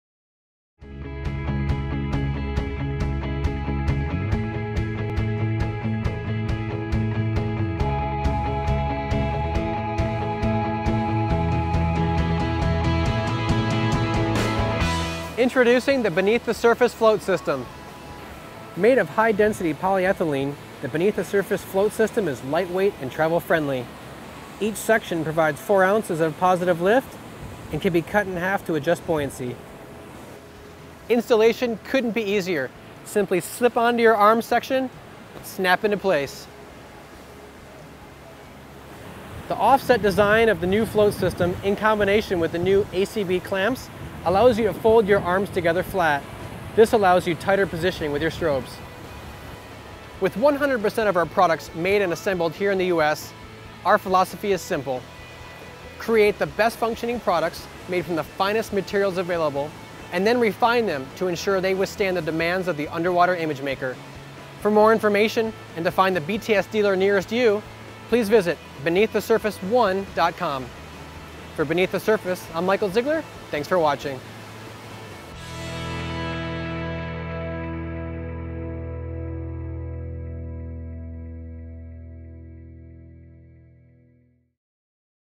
Beneath the Surface Buoyancy Floats System
Introduction to the Beneath the Surface Buoyancy Float system for the BtS Ball Jointed Arms